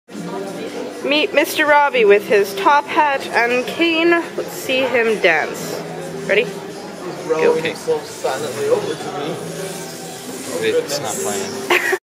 Robbie the Robot.mov
Dance Bot created with a Robix kit.